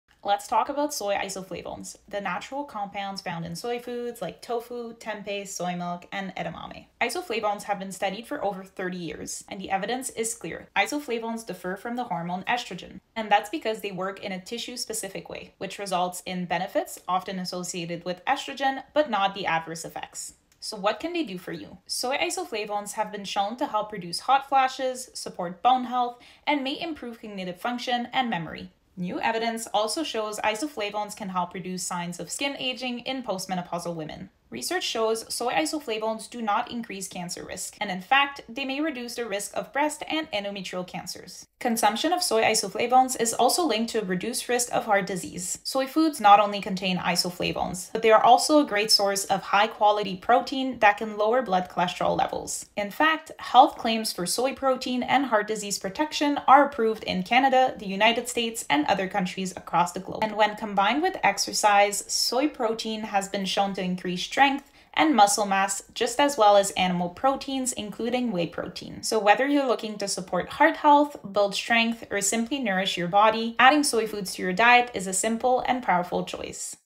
Soy foods like tofu, tempeh & soy milk offer more than protein.
Soy foods like tofu, tempeh & soy milk offer more than protein. They are a rich source of isoflavones which have been shown to:
- Reduce hot flashes
- Support heart, bone health & memory
- Lower cancer risk
- Benefit skin